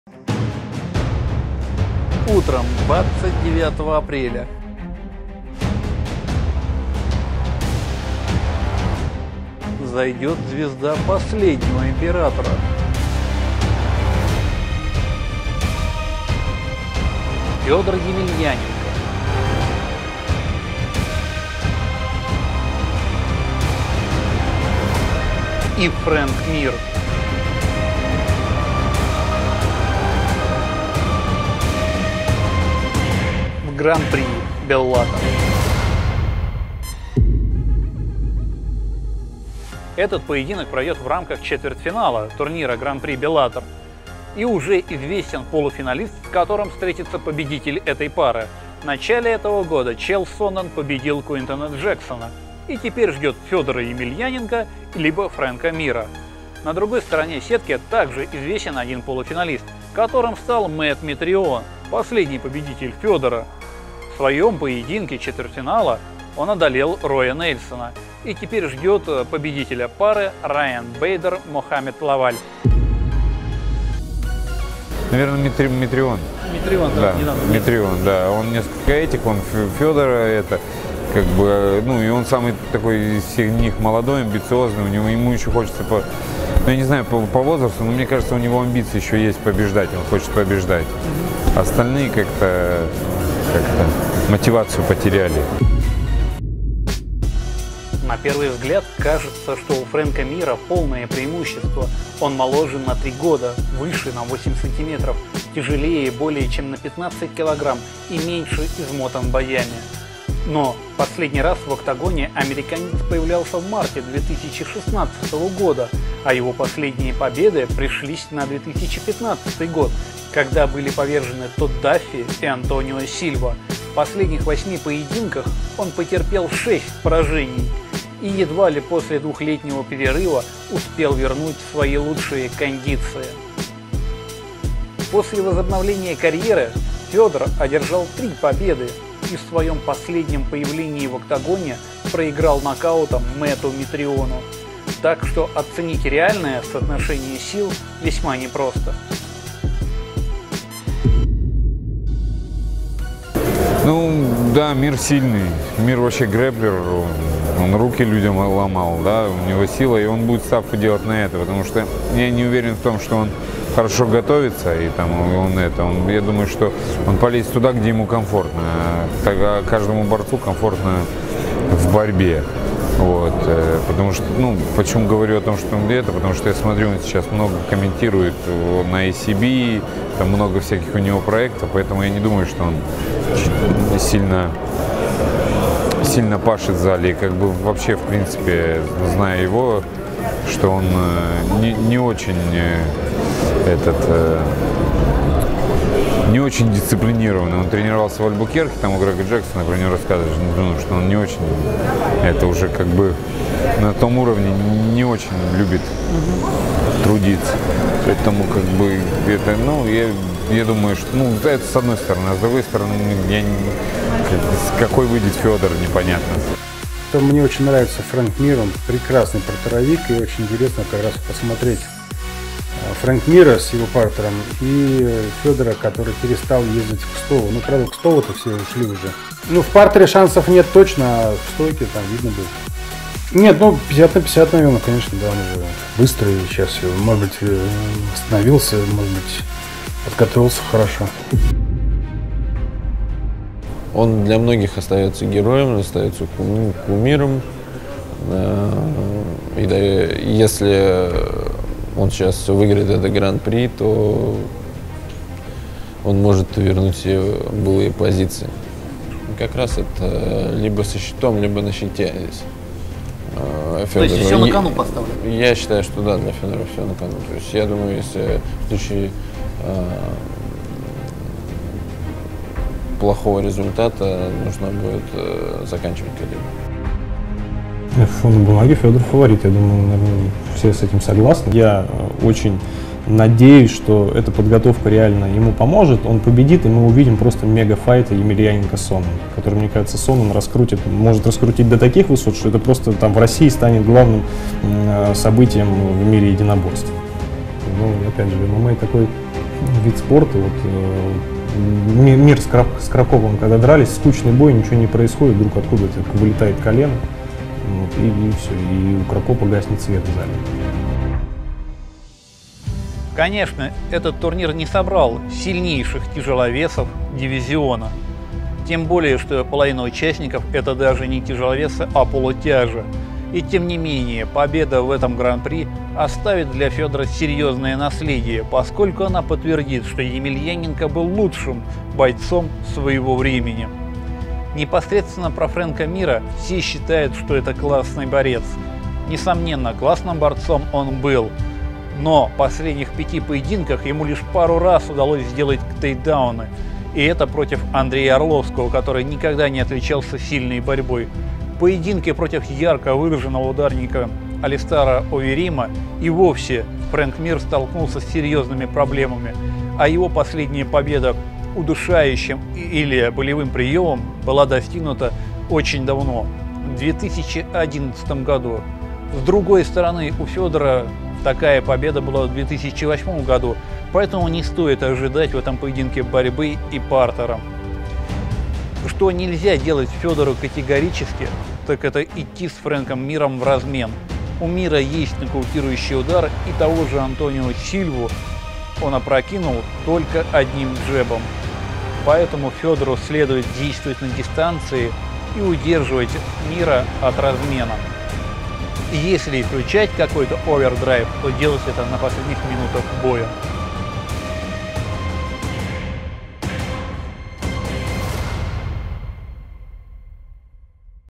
Война и Мир. Возможно, Федор дерется в последний раз
Встреча россиянина Федора Емельяненко и Фрэнка Мира в рамках четвертьфинала гран-при Bellator пройдет в воскресенье утром по московскому времени. Что ожидать от этого боя, и каков будет исход?